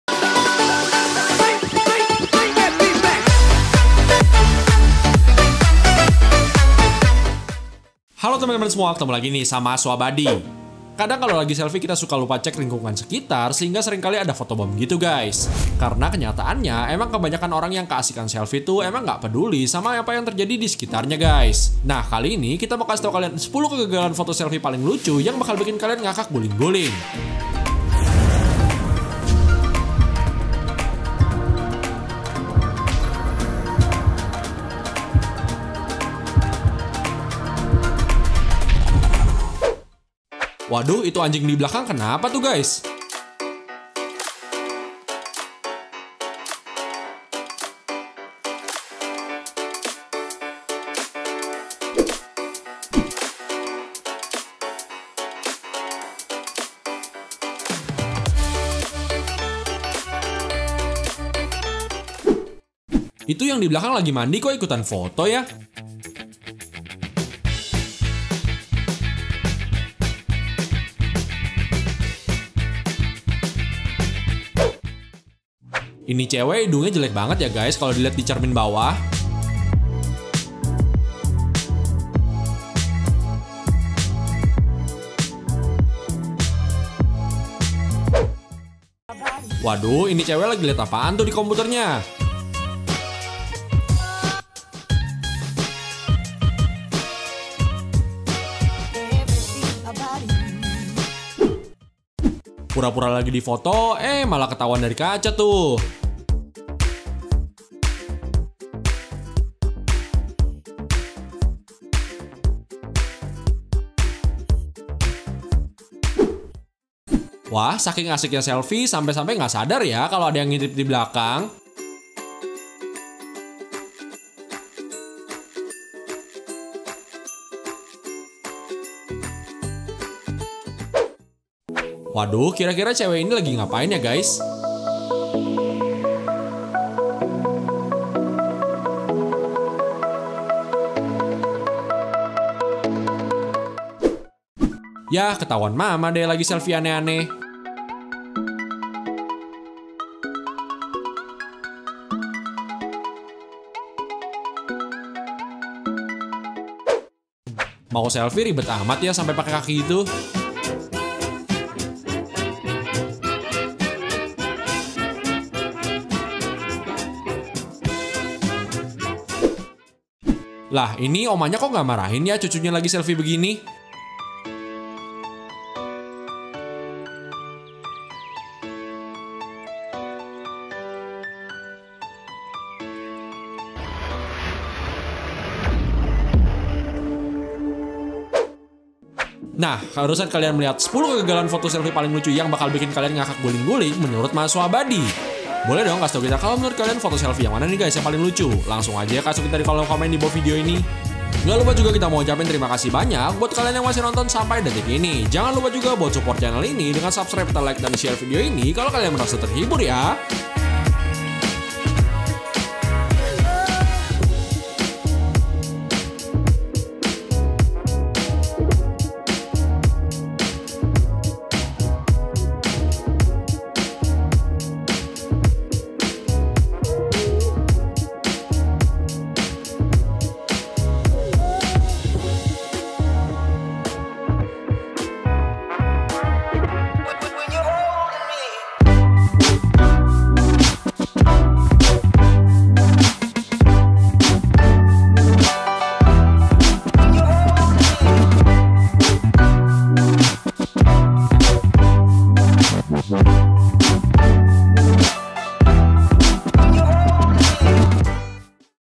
Inilah 10 Kegagalan SELFIE PALING MEMALUKAN yang Bakal Bikin Kalian Ngakak Guling-Guling !!!
Selfie emang sering banget dilakukan orang-orang terutama cewe-cewe guys. Tapi kalo lagi asik selfie tuh seringkali kita lupa diri sampe-sampe gak merhatiin lingkungan sekitar.

Nah beberapa orang ini tuh malah jadi jelek selfie nya karena ada orang atau objek aneh yang muncul di selfie tersebut. Dari pada basa basi langsung lihat aja yuk.